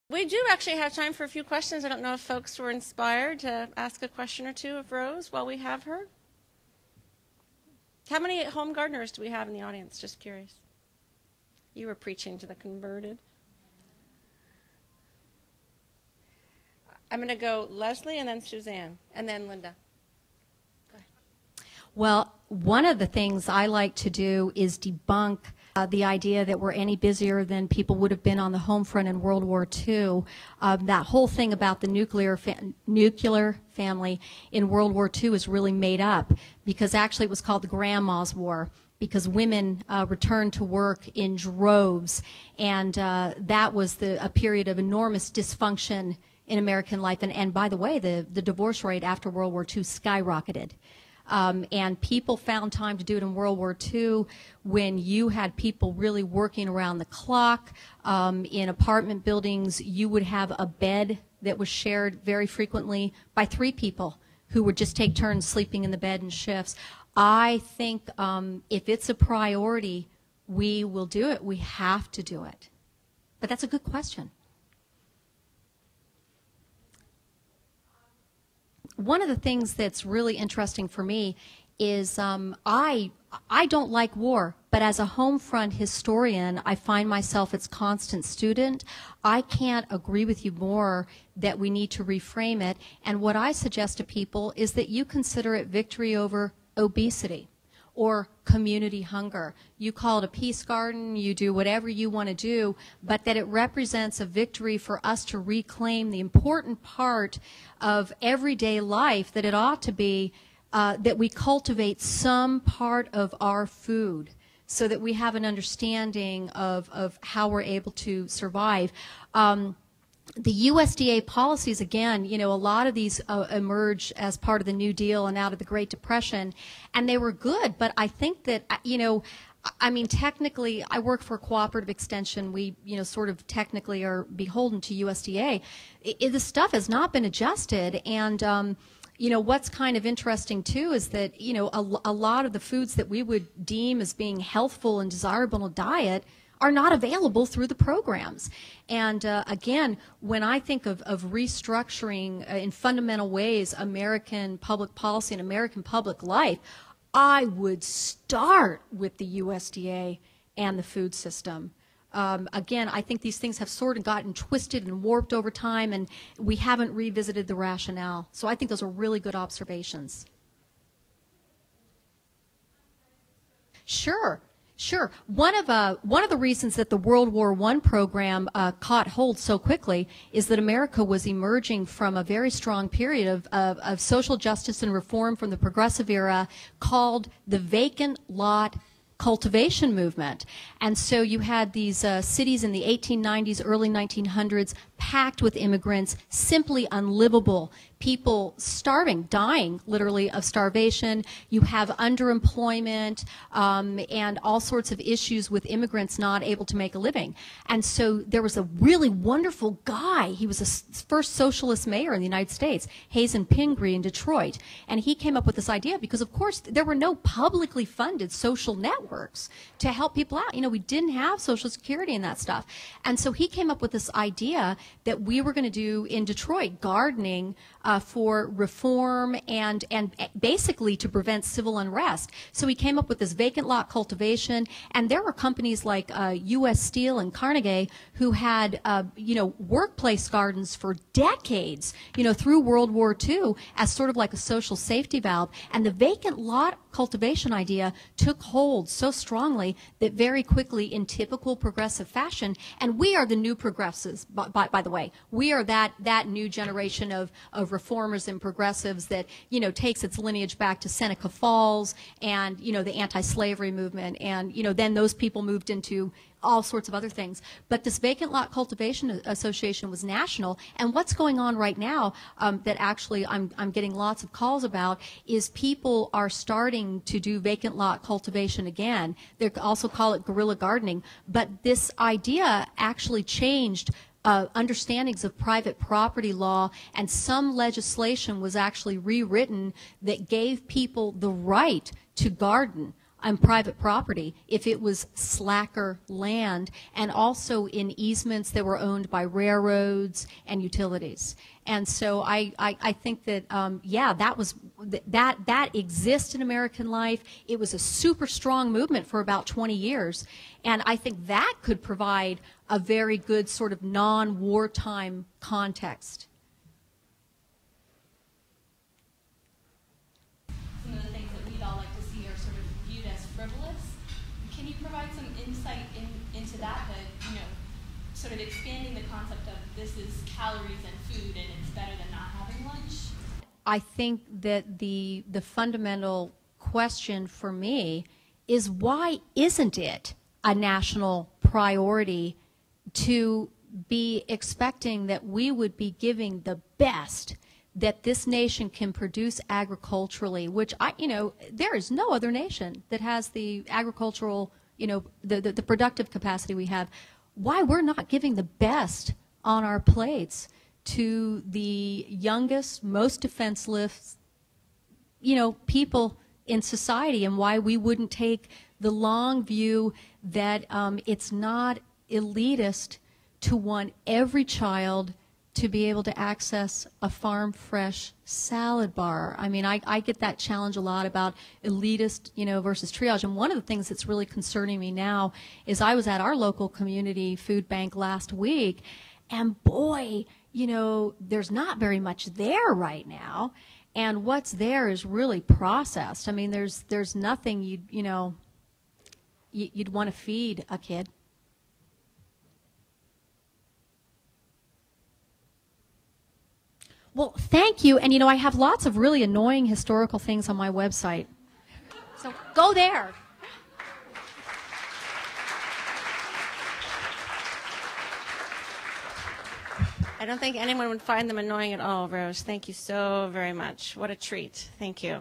Keynote Address - Part 5 of 6
Rose Hayden Smith helps Western Regional Farm to Schoolers review our history with school food and garden-based education as we move forward.